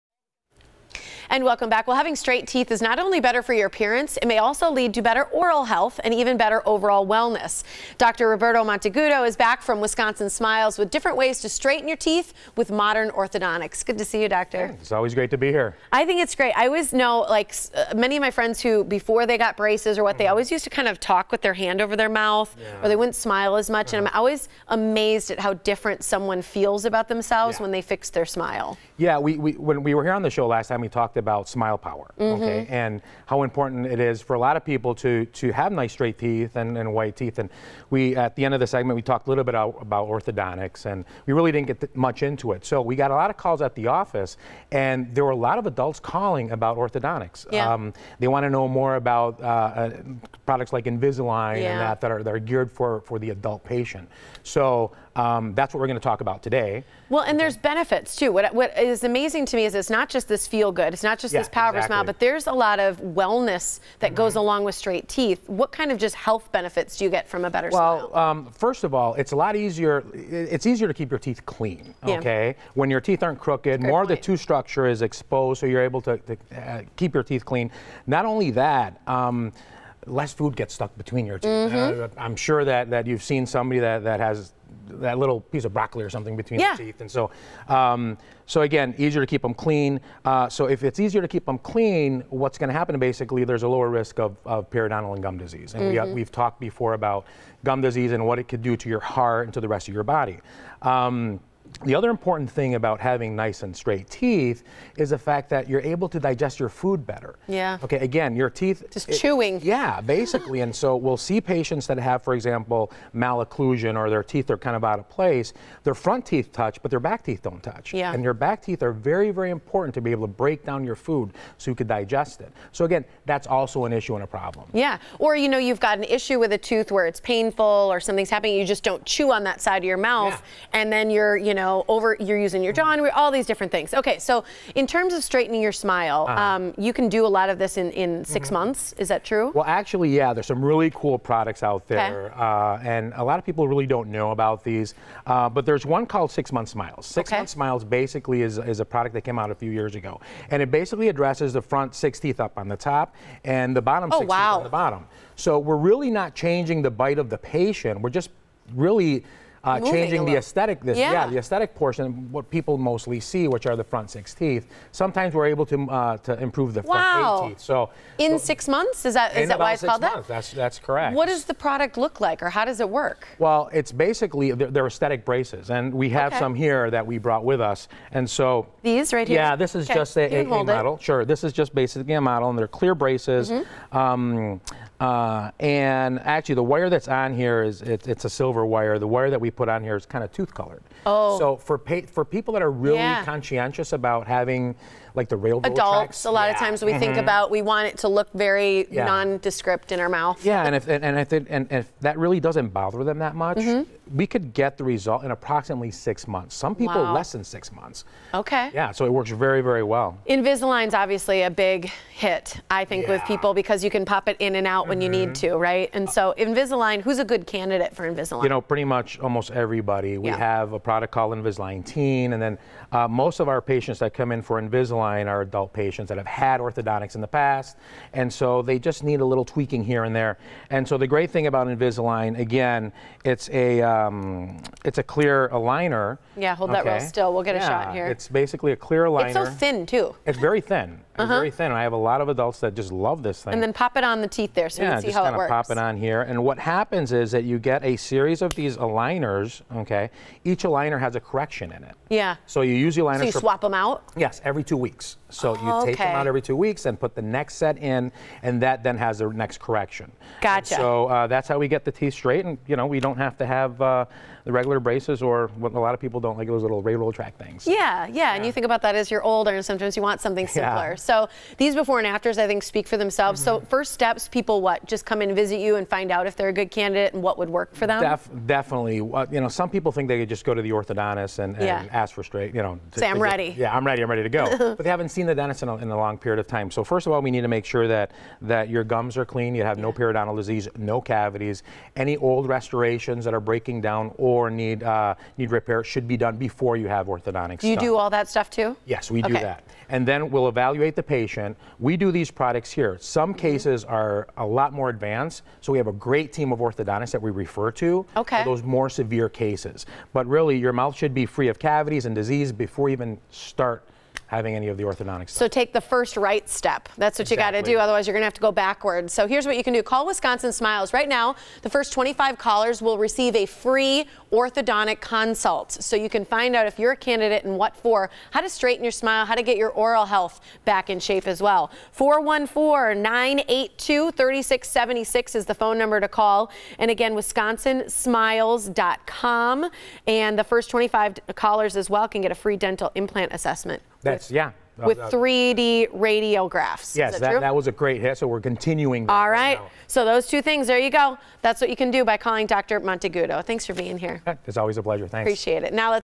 The Different Advancements in Modern Orthodontics
Like most dental treatments or procedures, orthodontics have come a long way in recent years. Having straight teeth is not only better for your appearance, it may also lead to better oral health and even better overall wellness. Joining us to discuss how you can get straighter teeth quicker and more discretely is Dr. Roberto Monteagudo.  The first 25 callers who contact WisconsinSmiles will receive a FREE orthodontic consult.